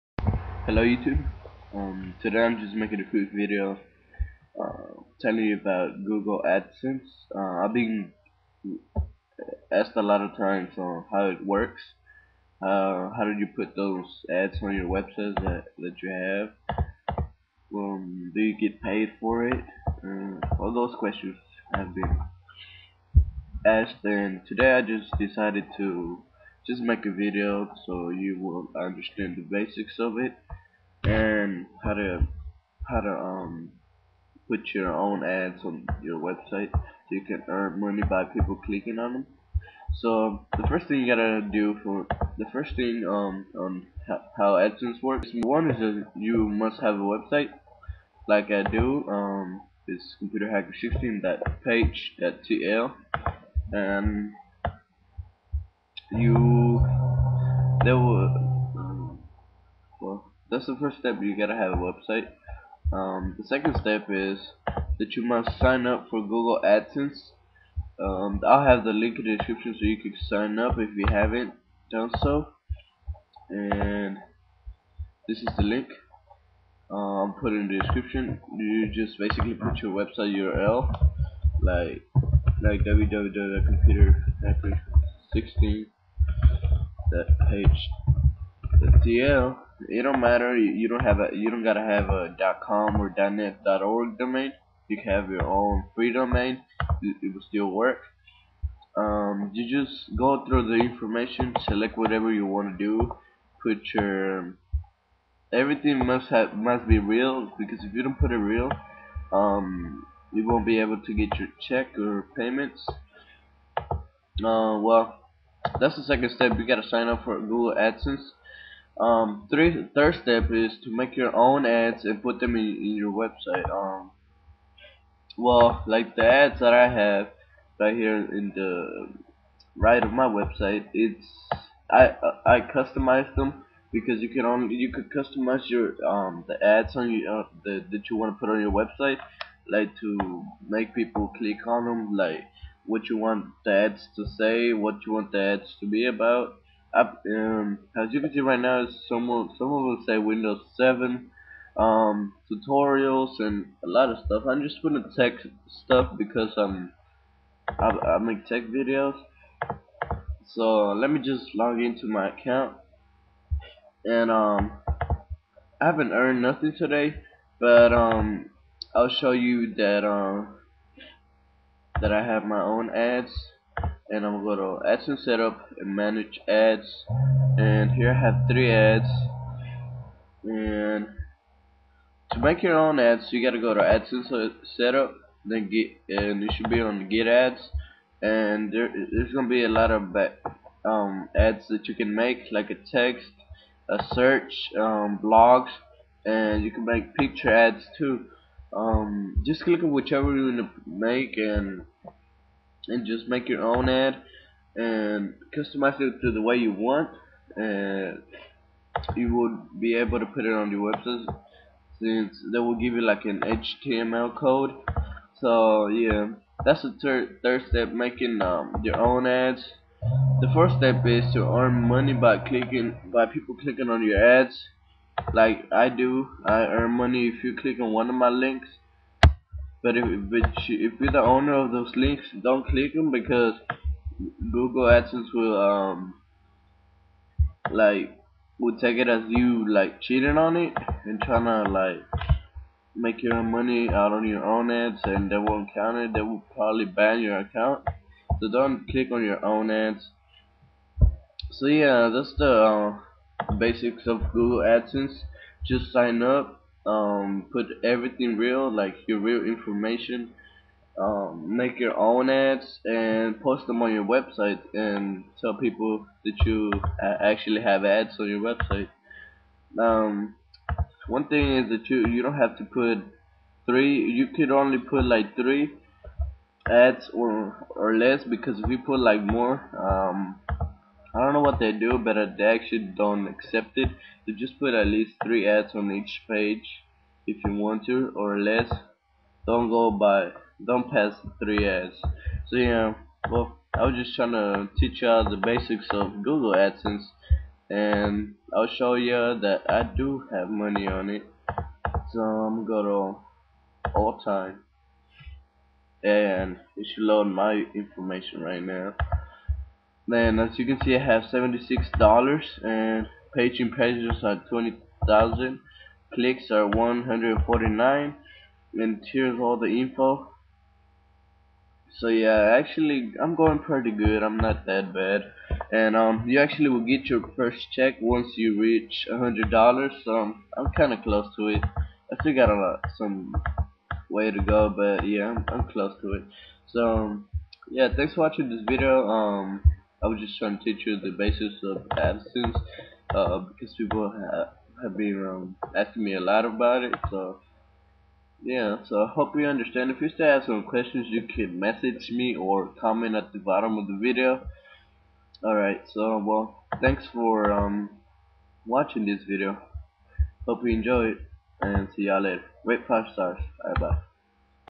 EARN MONEY FOR FREE WITH GOOGLE ADSENSE
How Adsense Works
Adsense is a program where you can get paid if
you have a website and you can advertise their ads
for you to get money.

1-Must Have a Website It doesn't matter if you dont have
a .COM, .ORG, OR .NET domain IT CAN BE A FREE DOMAIN.
2-Sign Up for Google Adsense

3-Make your own Ads and put
them in your website.

4-Earn moeny by people clicking
on your Ads.